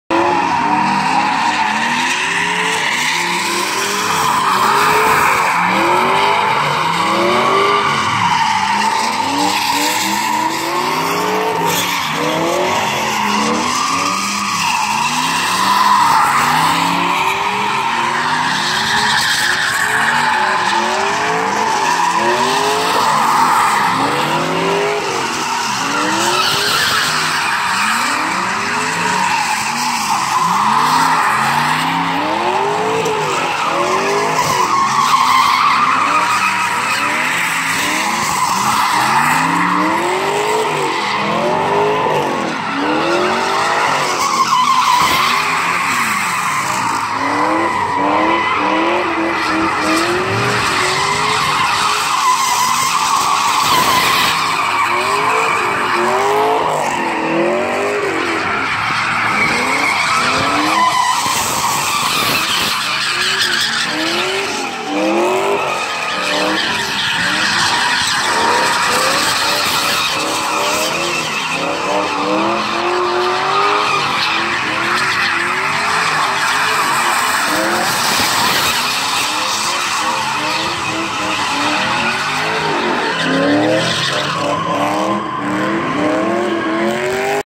CAMARO AND SILVERADO DONUTS
having some fun in mexico i dont know the info on the camaro but the truck is an all stock 2002 chevy silverado 1500 4.8